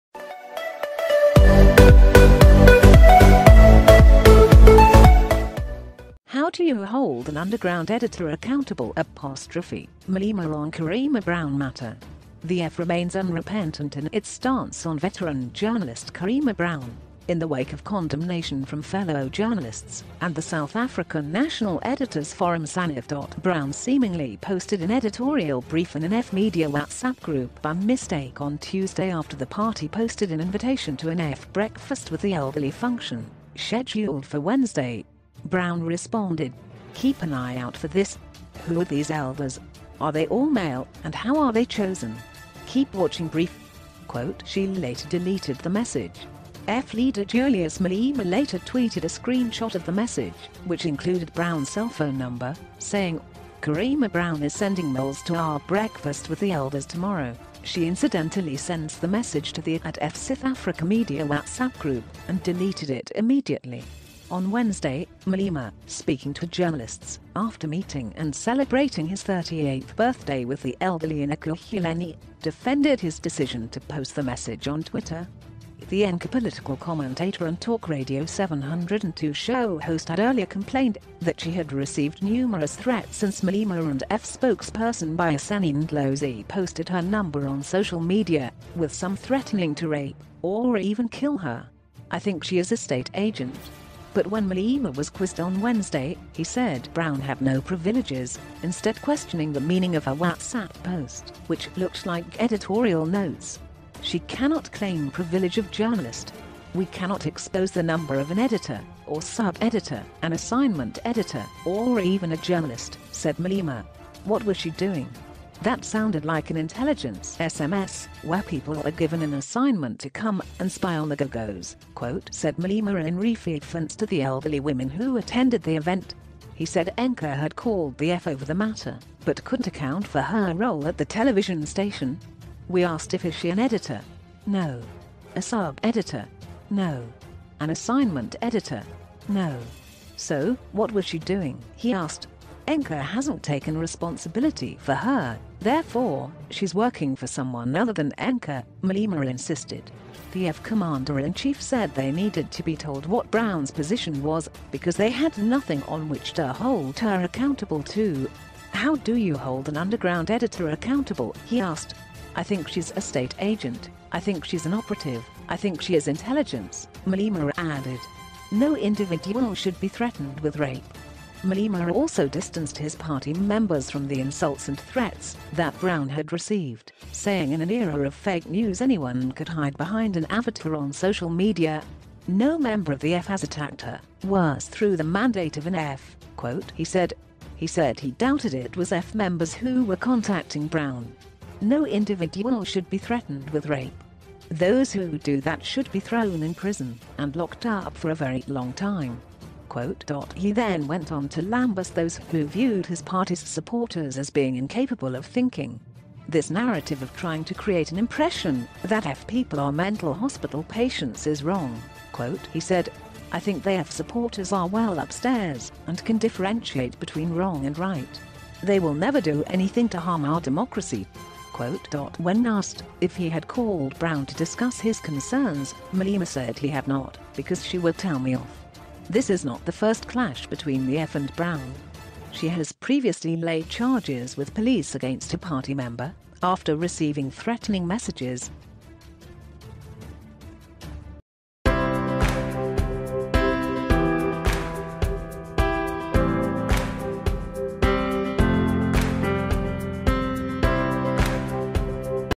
'How do you hold an underground editor accountable ' – Malema on Karima Brown matter
'How do you hold an underground editor accountable ' – Malema on Karima Brown matter. The EFF remains unrepentant in its stance on veteran journalist Karima Brown, in the wake of condemnation from fellow journalists and the South African National Editors' Forum (Sanef).
▬▬▬▬▬▬ஜ۩۞۩ஜ▬▬▬▬▬▬
🎥 MAY YOU WILL LIKE:

▬▬▬▬▬▬ஜ۩۞۩ஜ▬▬▬▬▬▬
🎥 MORE VIDEO BIOGRAPHY SOUTH AFRICA: